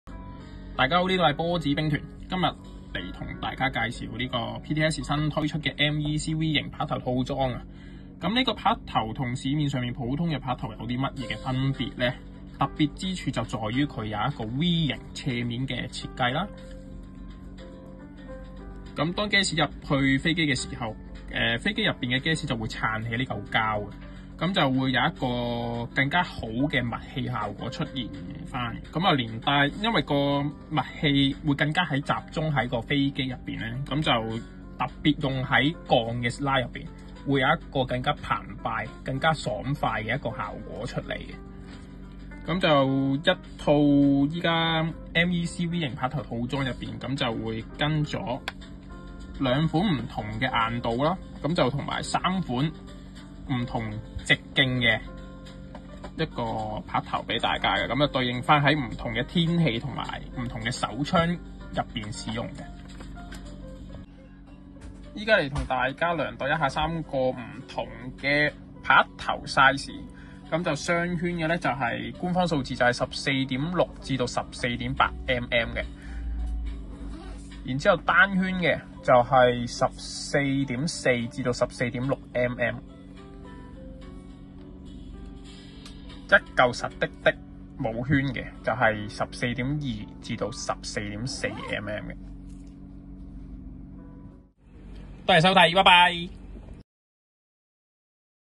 PTS MEC “New Product” MEC - V Piston Head For 14-15mm GBB Nozzle
Features:

Fast responsiveness on air-sealing
Designed to withstand high impact pressure.
Stabilized Performance.
Suitable for GBB Pistol with 14 to 15mm nozzle
Specification:

Material: Rubber
Weight: 8g (6pcs piston head with package)
Compatibility: GBB Pistol with 14 to 15mm nozzle